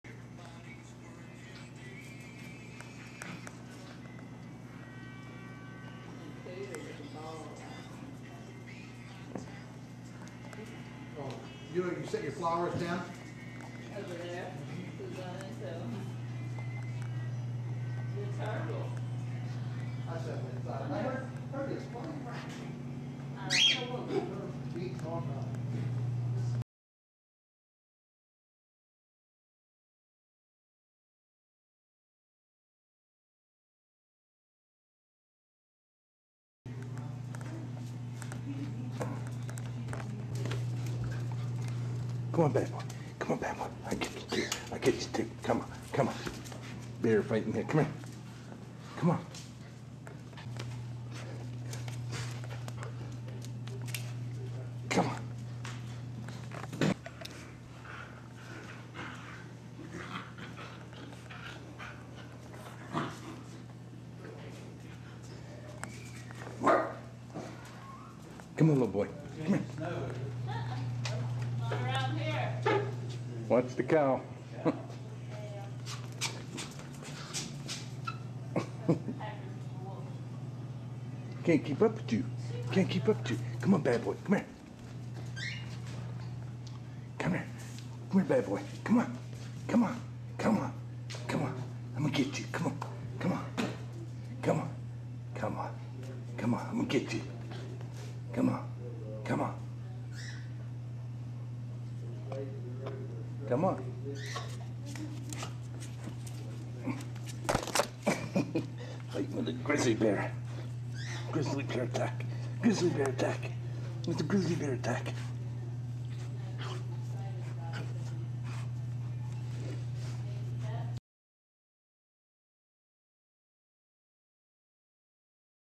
Grizly Bear Cub & Wolf Cub Playing - (c) Denmortube
Video by Dennis Crossland. ORIGINAL VIDEO of Lil' Bear and Tala playing in the Gift Shop of the Woodland Zoo. Note: In the middle of the clip there are pictures of these guys as they are 6 years later. THERE ARE NO VIDEO OF THEM 6 YEARS LATER BECAUSE THEY LEFT THE ZOO WHEN THEY WERE 5 MONTHS OLD AND I AM UNABLE TO CONTACT THE CURRENT OWNER.
 Also note that in the clip I keep referring to the bear as a "boy", it's actually a girl.